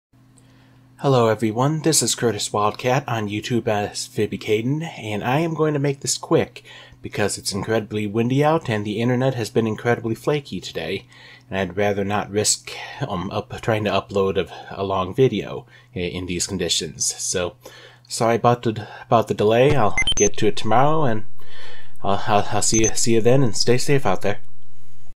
Status Update for November 26, 2019
Videos are on hold for a day or so, and I explain why in a way that illustrates why I don't do commentary without the aid of Notepad.